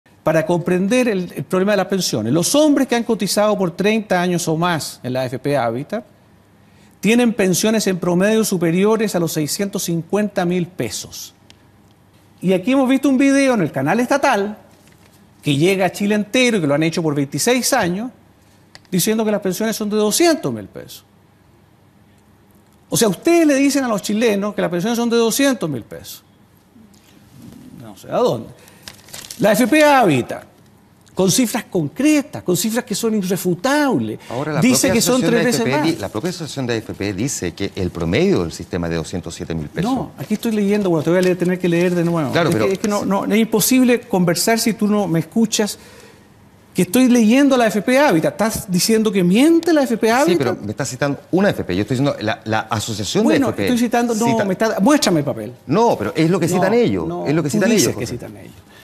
José Piñera vs. Juan Manuel Astorga sobre sistema de capitalización individual
José Piñera vs. Juan Manuel Astorga sobre el sistema de capitalización individual en TVN.